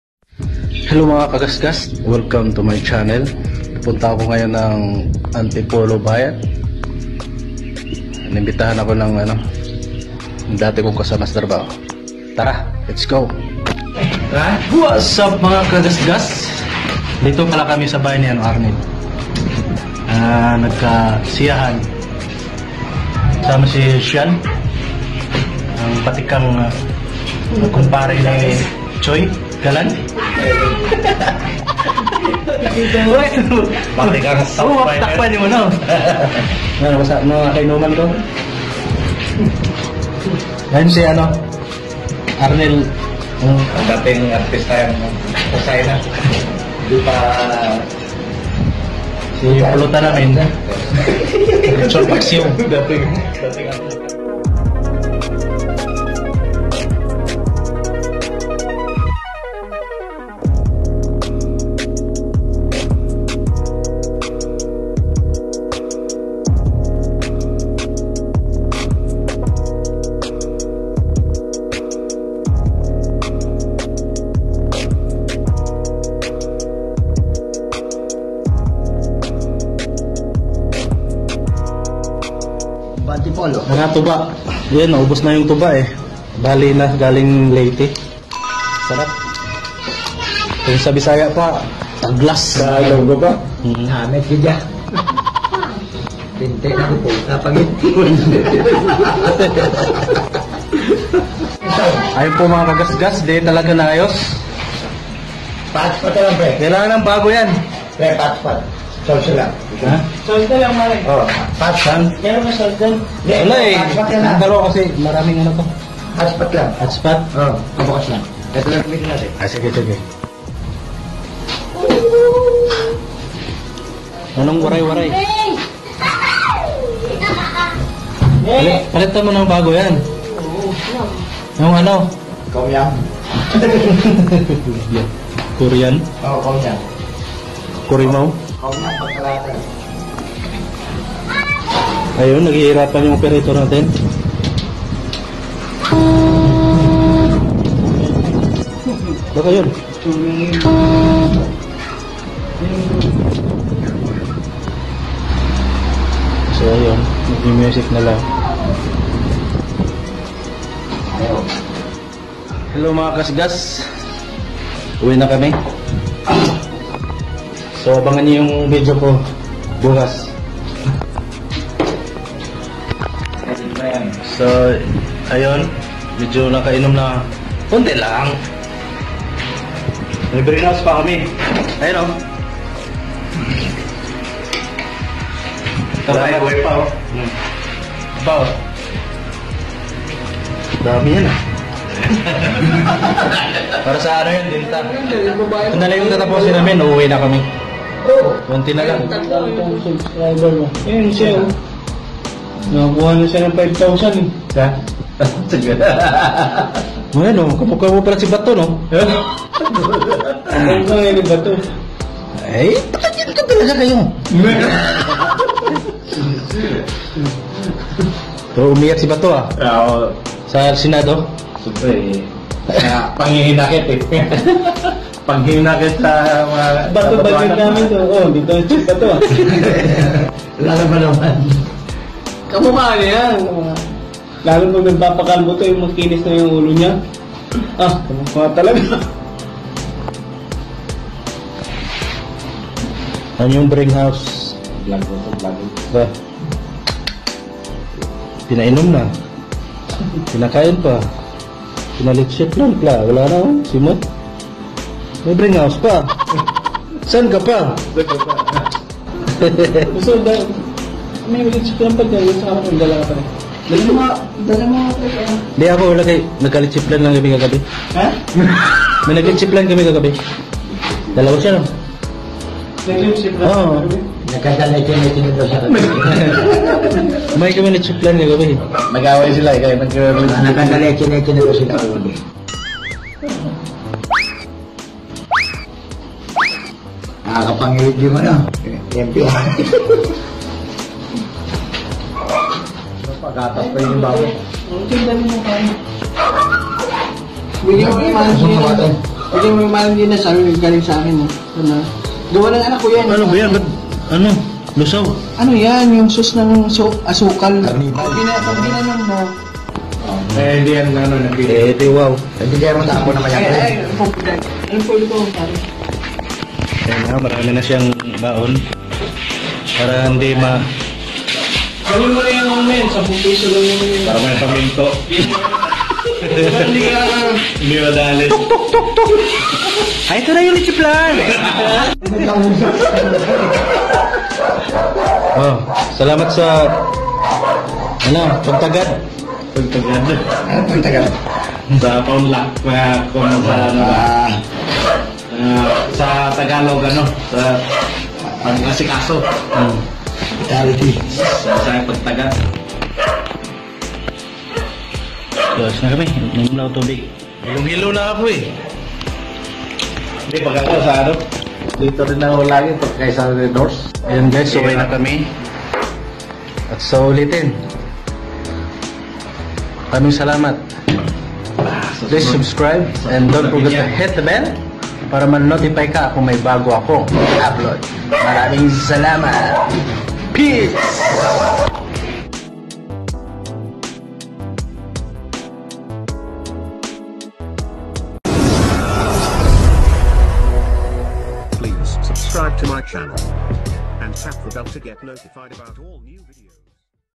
Reunion 8 years bago Naulit | kasama si Bato
Walong taon bago naulit ang ganitong kasayahan na may konting inuman..